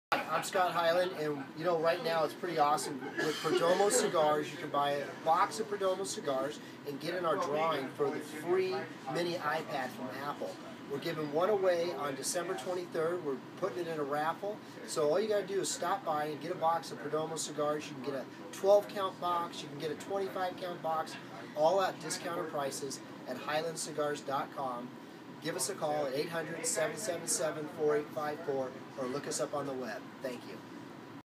FREE! Apple iPad Mini Perdomo Cigars
Buy any box of perdomo cigars and be entered in to a drawing for a free iPad Mini on December 23rd